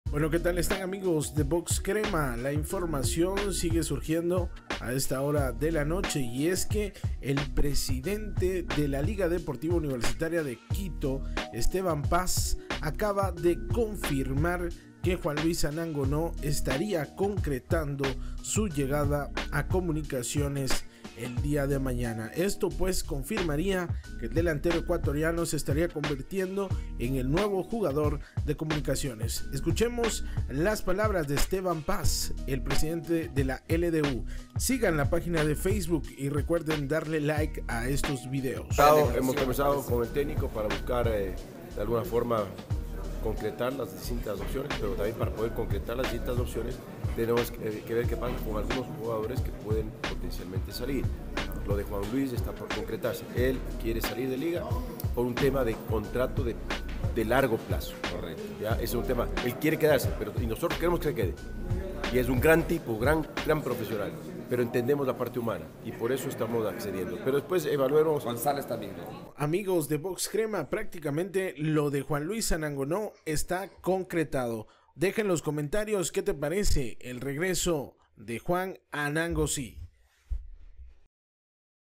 NO ES RUMOR ANANGONO REGRESA
YA NO ES UN RUMOR ANANGONO REGRESA

El presidente de la LDU confirma que mañana se concretara el tema.